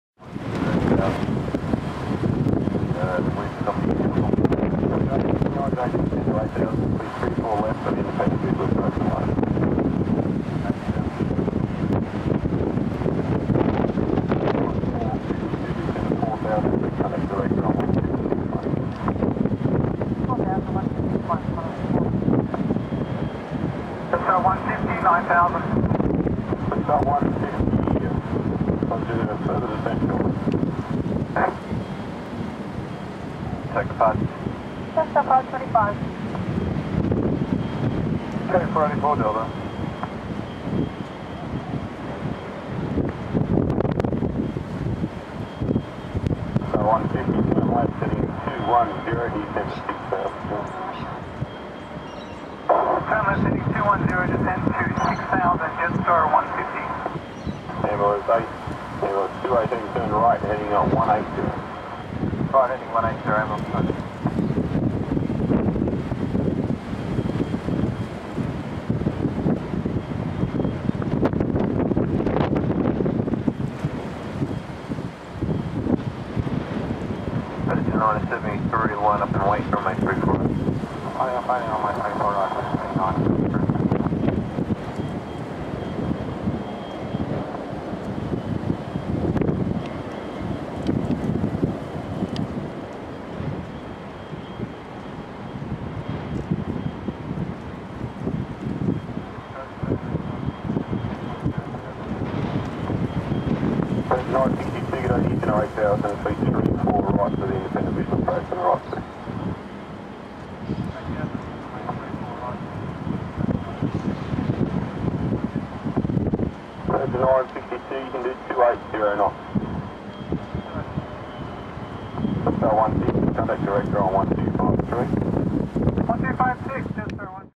Aussie 1 Airbus A340 crosswind landing
Aussie One - Airbus A340 operated by the Department of Defense carrying Defense force personnel including those of the Royal Australian Airforce.